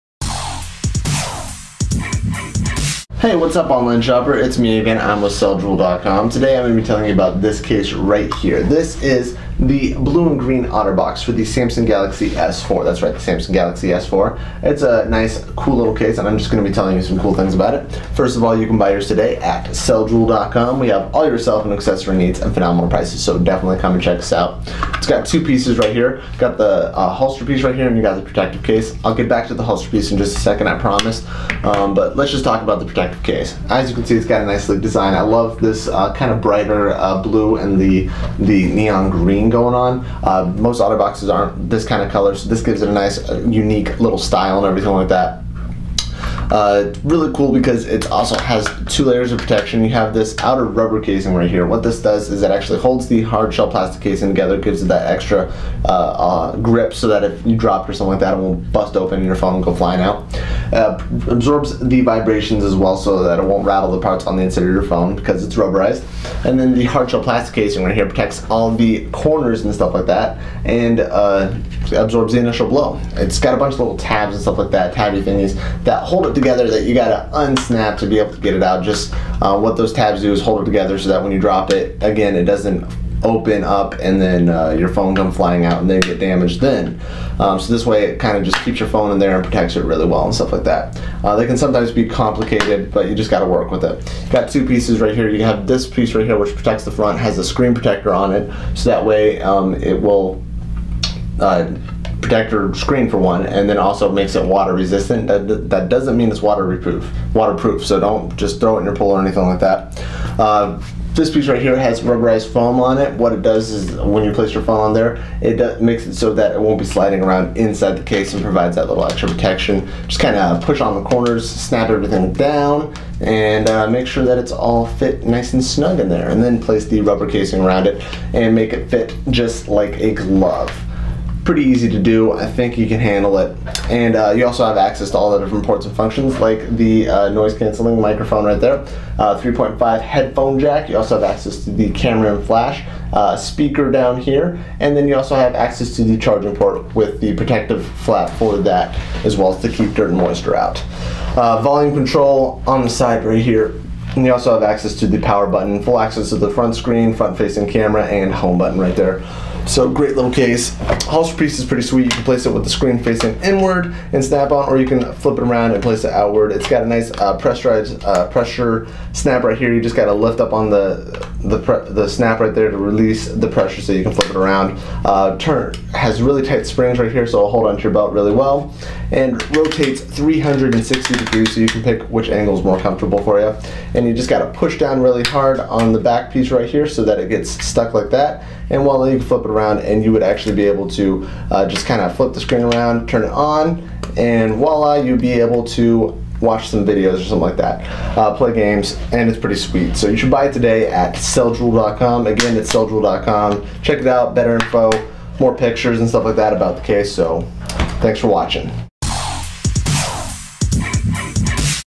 CellJewel.com - Samsung Galaxy S IV/ Galaxy S4 Blue Otter Box
The OtterBox Defender Series offers an exclusive three-layer design that keeps information and investments safe. No other case can withstand this degree of rough treatment. The Defender Series provides superior protection for a smartphone allowing complete interaction through the case.